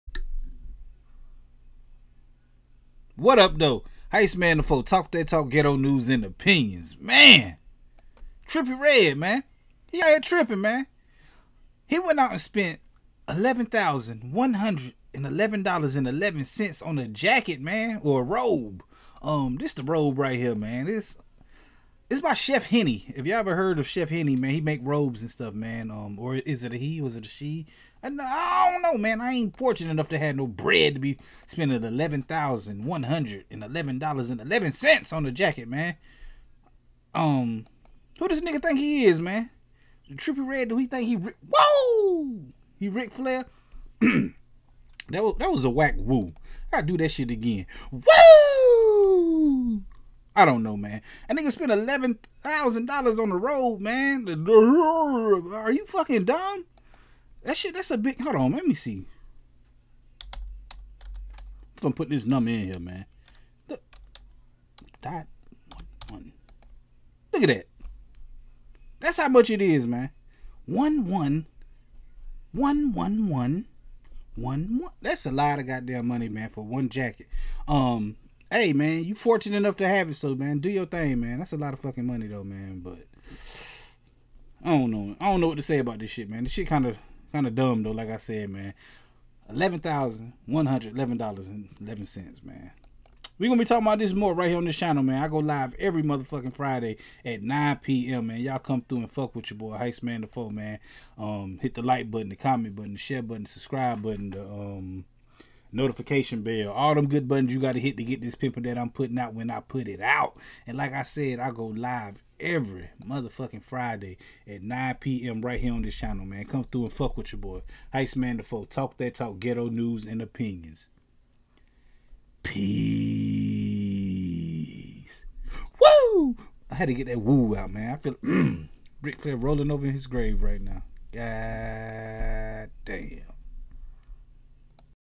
Trippie Redd Spends $11,111.11 on Fancy Robe...WHY???
Rapper Trippie Redd spends $11,111.11 on a robe designed by Chef Henny.